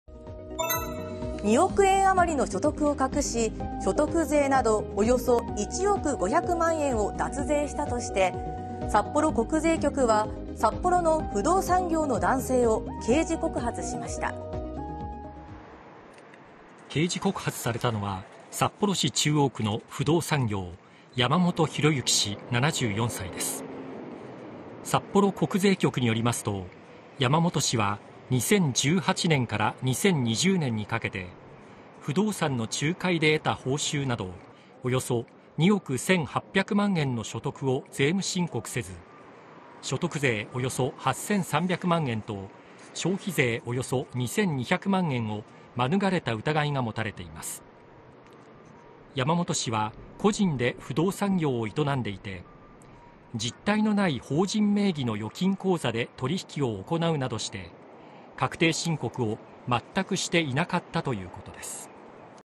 ２億円余りの所得を隠し１億円以上脱税した疑い、札幌市の不動産業の74歳男性 実態のない法人名義の預金口座で取引
２億円余りの所得を隠し所得税などおよそ１億500万円を脱税したとして、札幌国税局は札幌市の不動産業の男性を刑事告発しました。2023年05月31日(水) 17時31分 更新